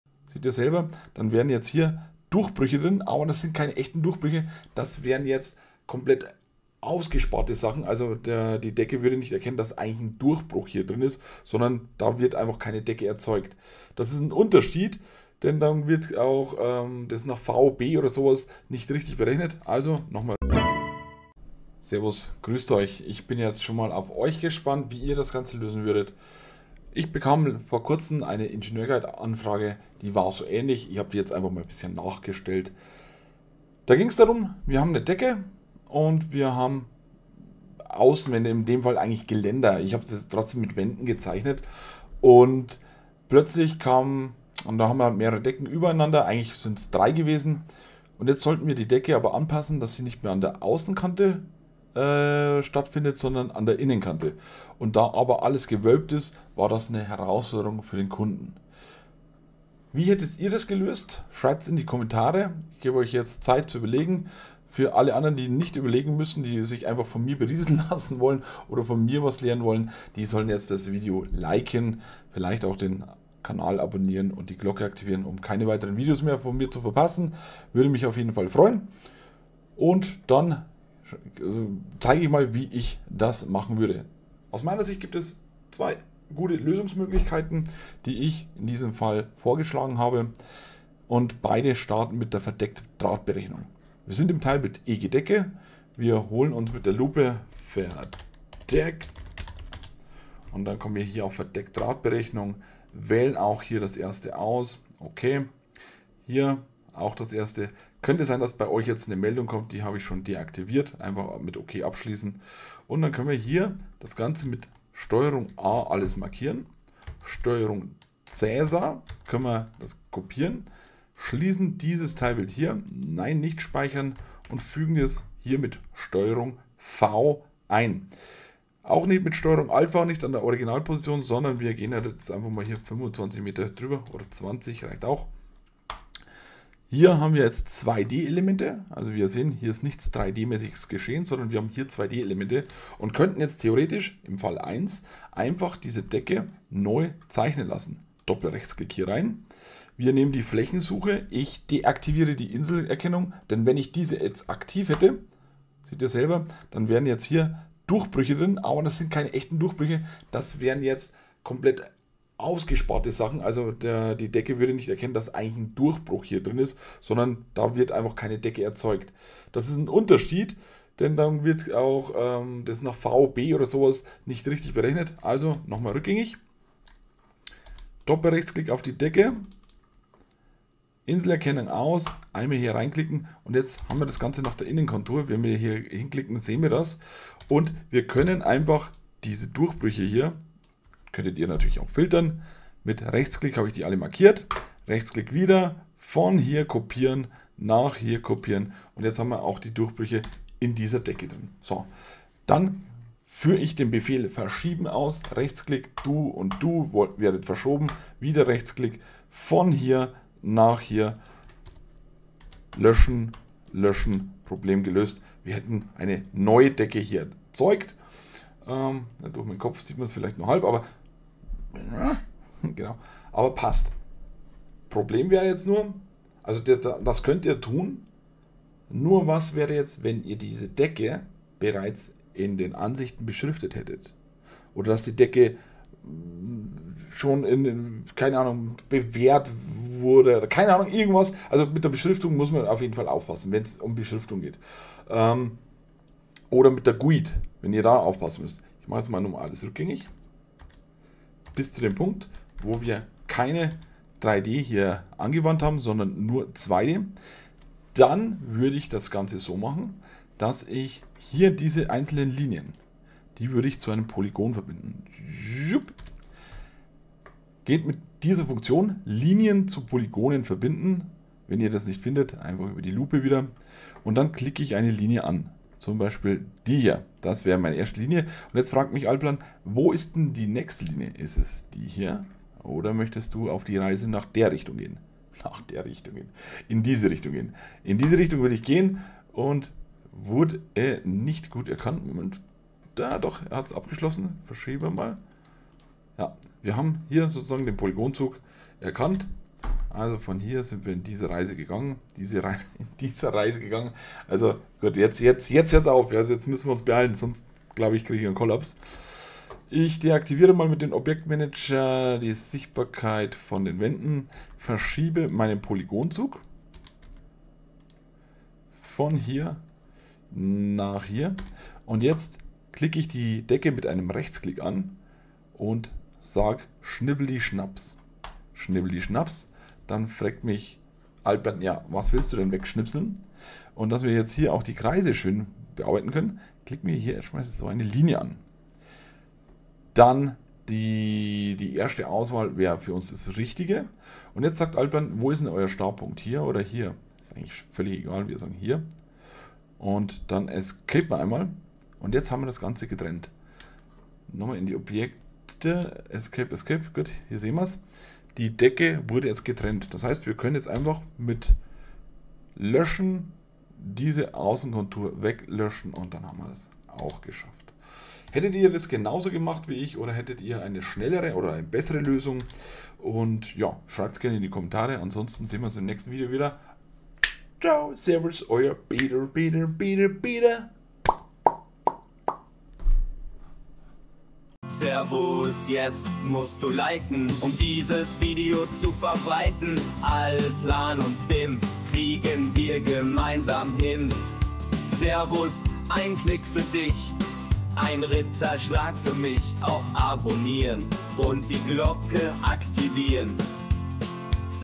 Komplexe Decken nachträglich zuschneiden, oder bearbeiten
Manchmal hat man es geschafft und in Allplan eine sehr komplexe Decke erzeugt, mit Rundungen, Durchbrüchen, Einbauteilen und vielem mehr. Doch was wäre, wenn wir die Decke plötzlich zuschneiden müssten? Bleibt uns dann das mühsame Punkt-für-Punkt-Modifizieren erspart, oder gibt es einfache Lösungen, mit denen wir die Decke in die richtige Form bringen können? In meinem neuen Video zeige ich euch, wie das geht. Wie würdet ihr es machen? Ich stelle euch kostenlos meine zwei Lösungsansätze vor und würde mich im Gegenzug über ein Like freuen.

00:00 Vorschau
00:21 Aufgabenstellung
01:30 Verdeckt Drahtberechnung
02:15 Lösung 1: neue Decke
03:45 Mögliches Problem von Lösung 1
04:20 Lösung 2: Decke zuschneiden
06:24 Liken, Abonnieren, Kommentieren